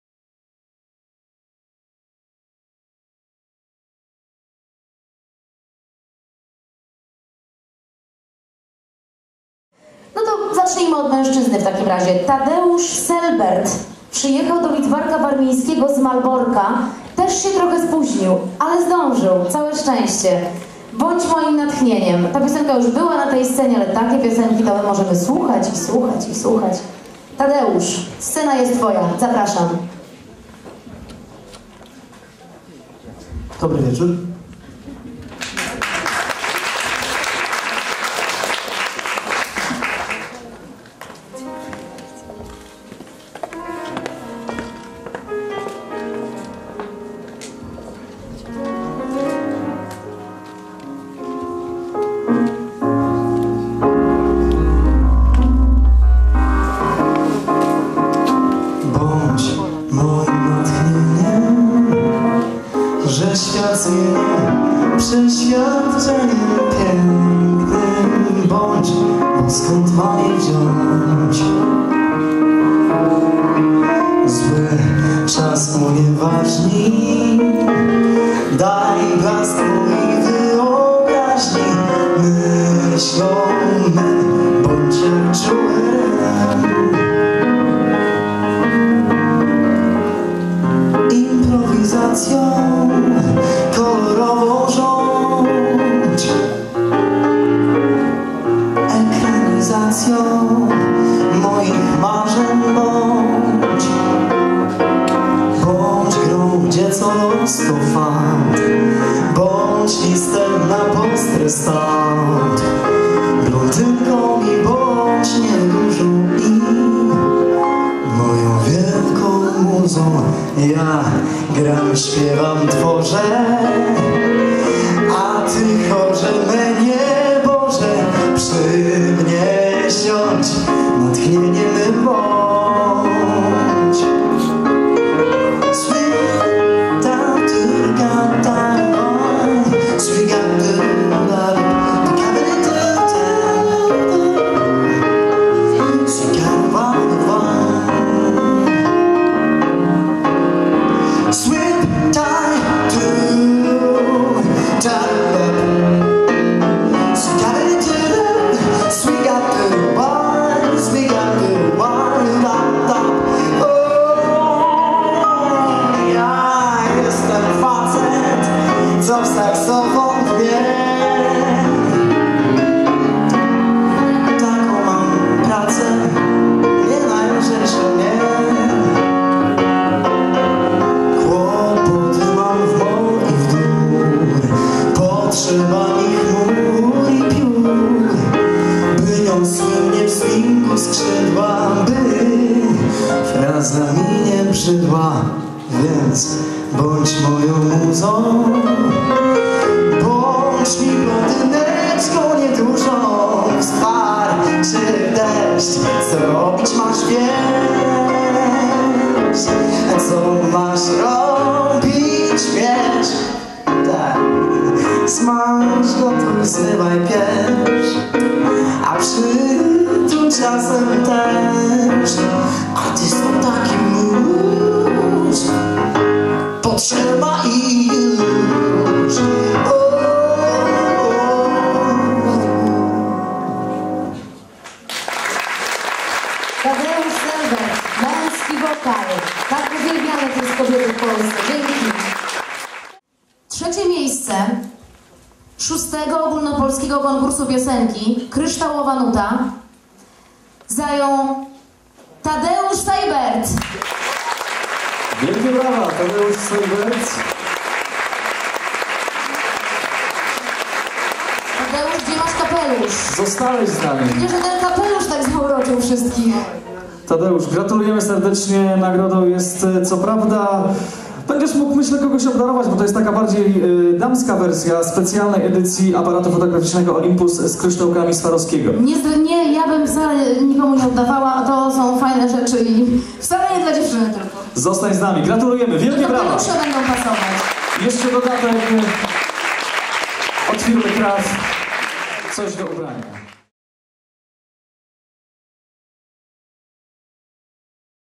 Tadeusz Seibert - Kryształowa Nuta 2010 - Lidzbark Warmiński
Tadeusz Seibert  zajął III miejsce  w Ogólnopolskim Przeglądzie Piosenki Kryształowa Nuta 2010 w Lidzbarku Warmińskim.
W jury konkursowym zasiedli:
Elżbieta Skrętkowska
Elżbieta Zapendowska
Grzech Piotrowski